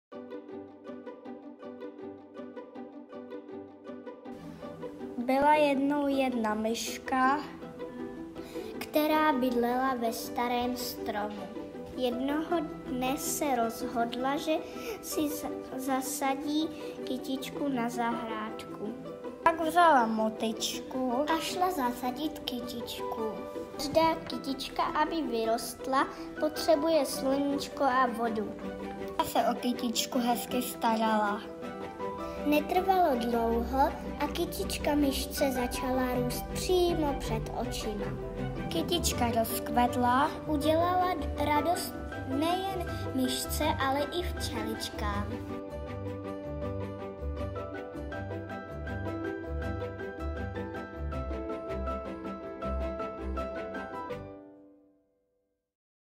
ZUŠ Unhošť_Myška Anežka - animace
První animace nejmenších studentů Výtvarného oboru ZUŠ Unhošť